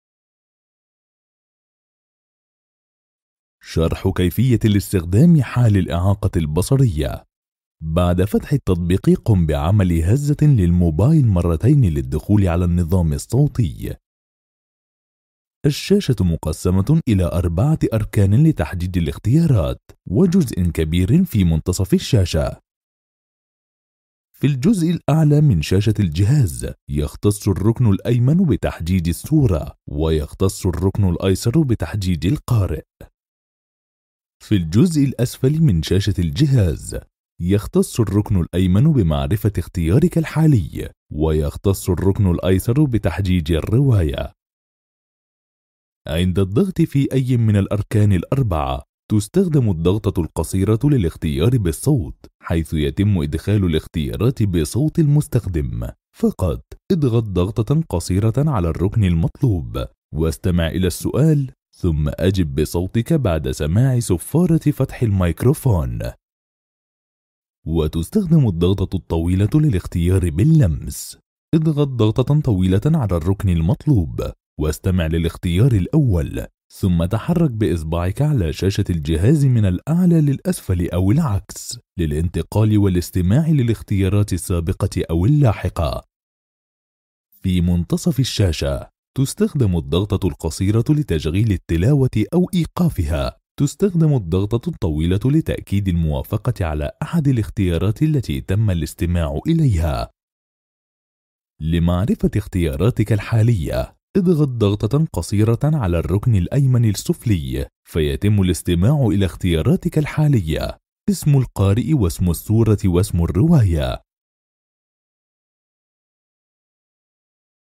Audio guide-Mp3 Quran
Audio guide for Mp3 Quran app blind mode